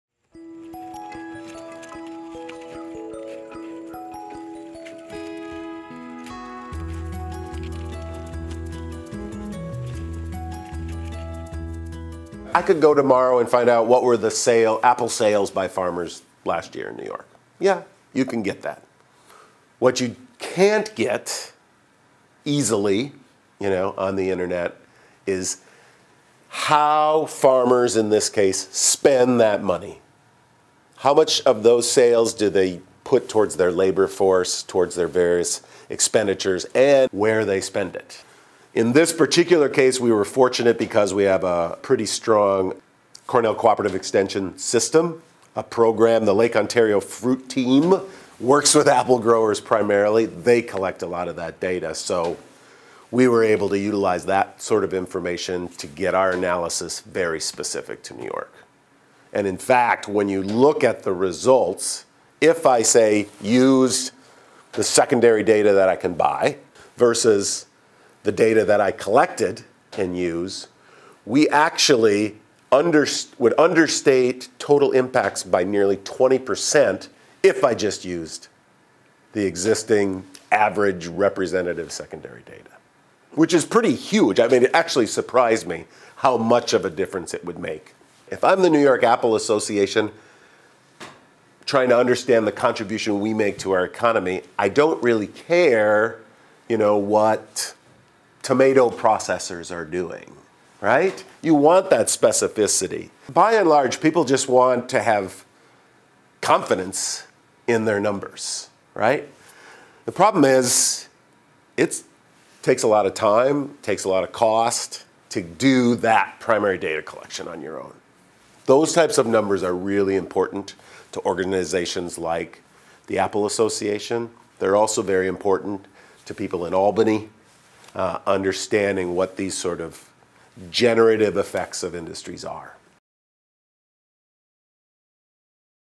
Study reveals NYS apple industry’s true economic impact
The New York apple industry's boost to the state economy is 21 percent greater than traditional models have suggested, according to a new study published by Todd Schmit, an associate professor at the Charles H. Dyson School of Applied Economics and Management. Schmit's research used data collected by the Cornell Cooperative Extension.